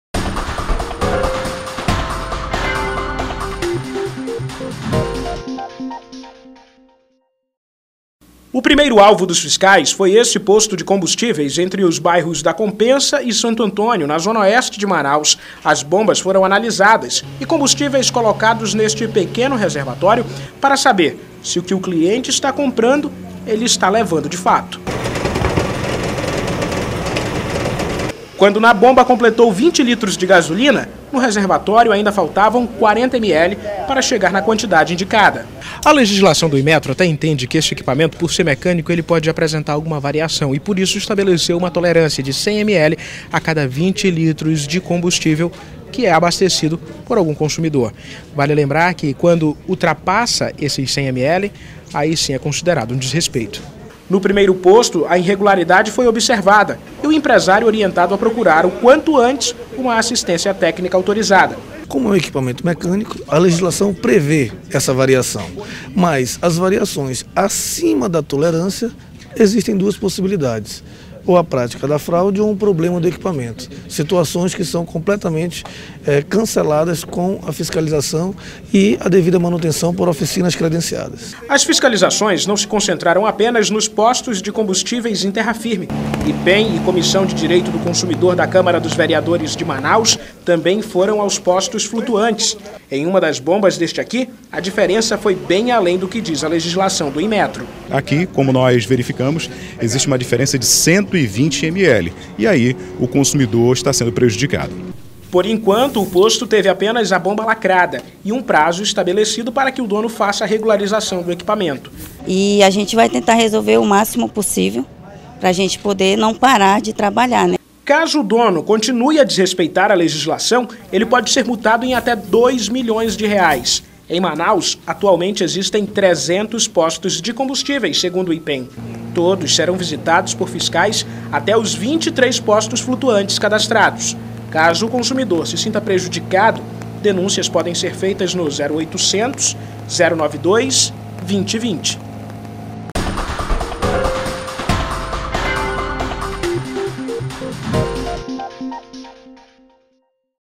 Posto de combustível na orla de Manaus é lacrado em operação do Ipem
O Instituto de Pesos e Medidas do Amazonas (Ipem/AM) deu início na manhã de hoje, 23, a fiscalização de 27 postos de combustíveis na capital, incluindo os flutuantes, também conhecidos como pontões. Durante a operação, que também será realizada em alguns municípios do interior, um posto localizado na orla de Manaus foi lacrado por vender menos combustível que o valor indicado na bomba.